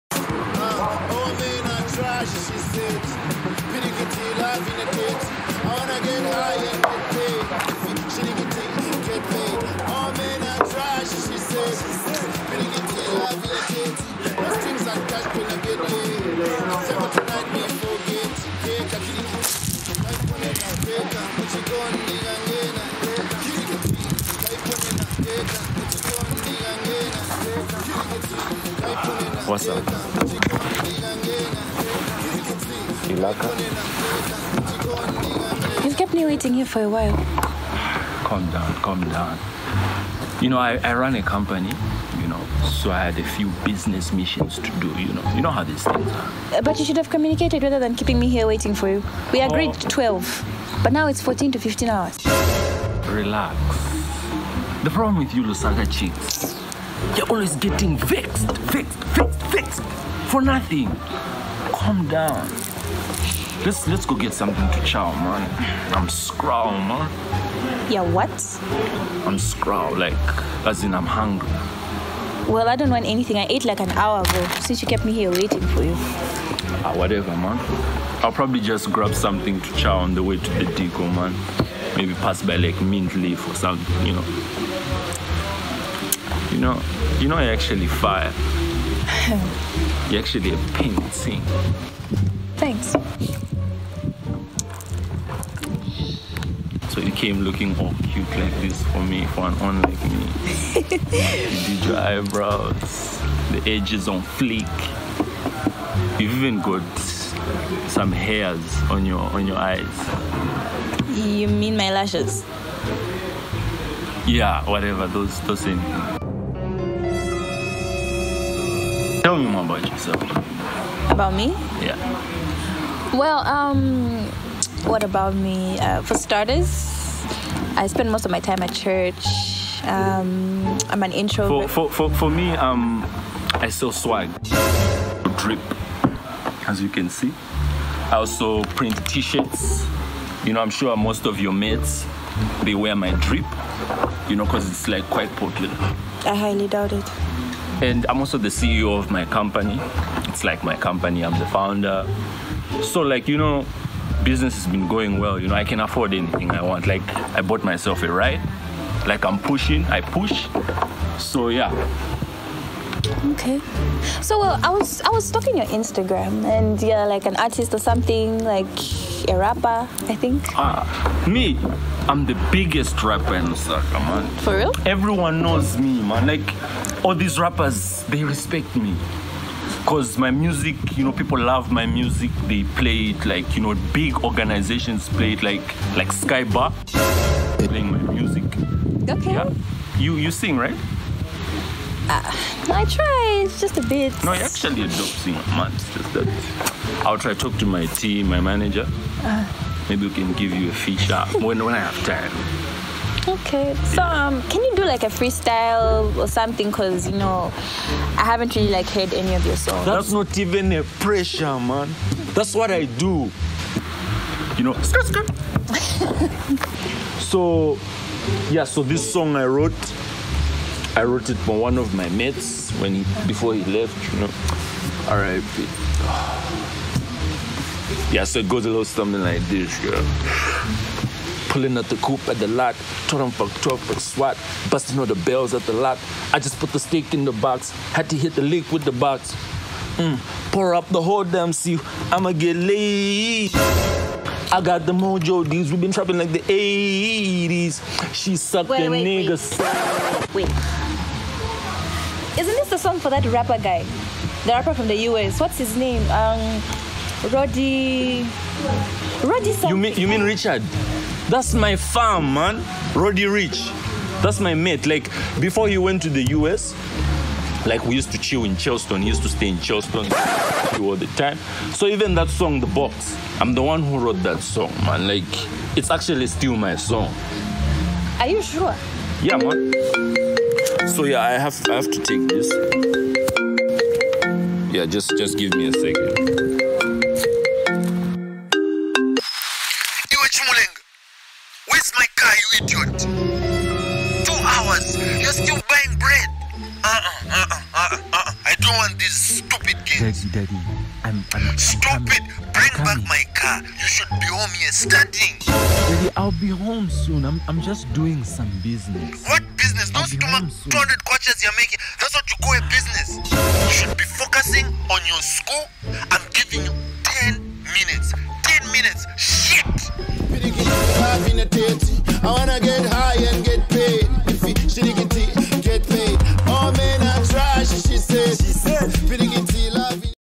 Lusaka Boy Starter Pack || Skit
A visual representation of how LSK boys behave lol.
Directed by Mkasamwa
Written by Marie Ndhlovu and M. Kasamwa